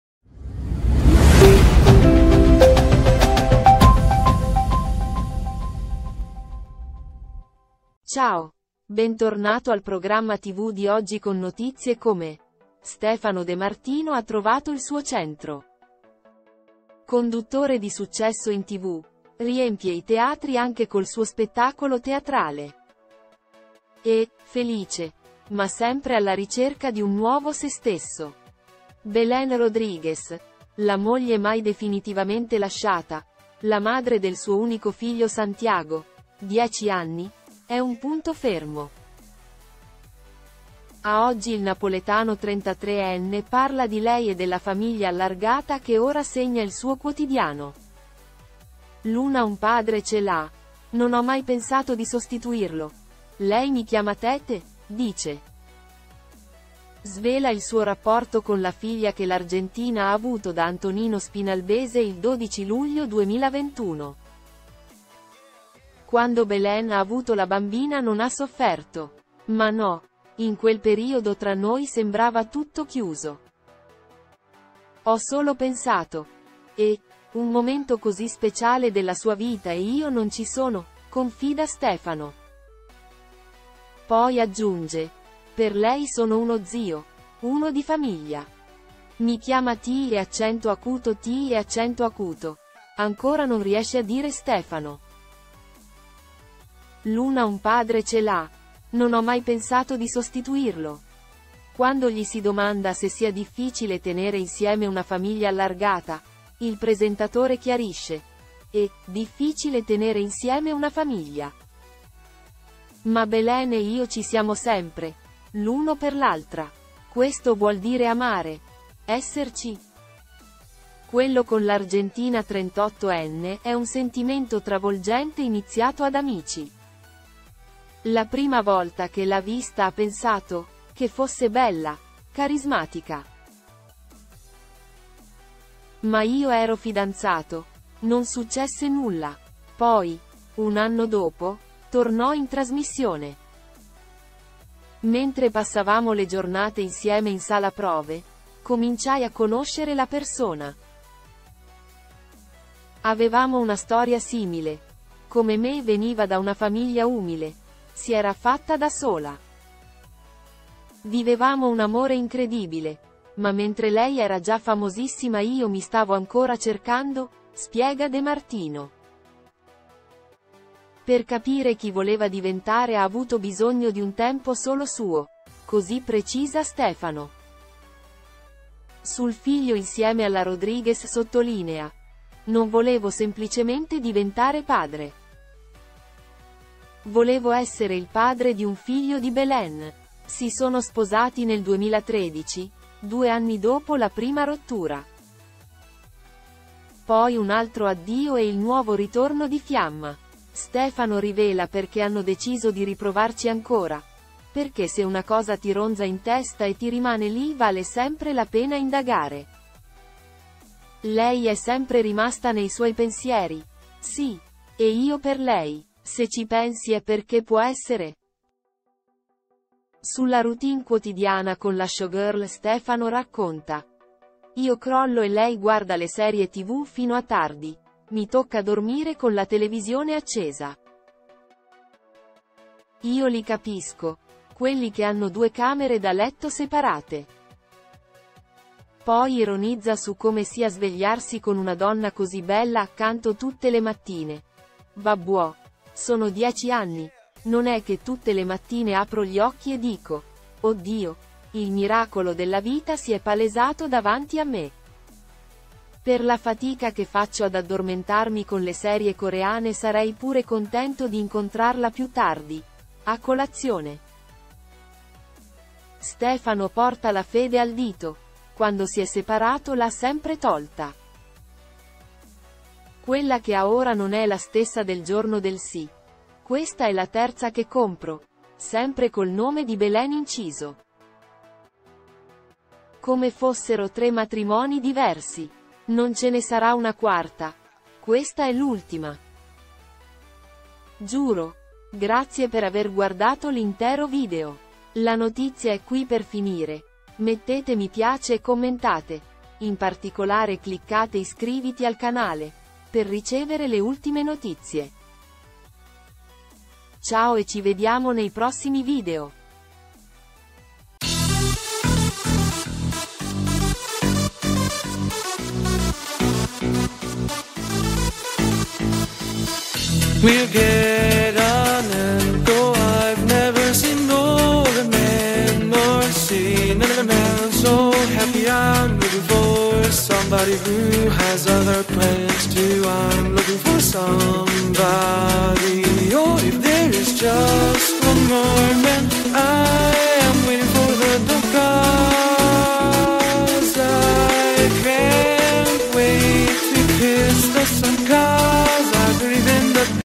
Stefano De Martino parla della famiglia allargata con Belen
Stefano De Martino ha trovato il suo centro. Conduttore di successo in tv, riempie i teatri anche col suo spettacolo teatrale. E’ felice, ma sempre alla ricerca di un nuovo se stesso. Belen Rodriguez, la moglie mai definitivamente lasciata, la madre del suo unico figlio Santiago, 10 anni, è un punto fermo. A Oggi il napoletano 33enne parla di lei e della famiglia allargata che ora segna il suo quotidiano. “Luna un padre ce l’ha, non ho mai pensato di sostituirlo, lei mi chiama Tèté”, dice. Svela il suo rapporto con la figlia che l’argentina ha avuto da Antonino Spinalbese il 12 luglio 2021.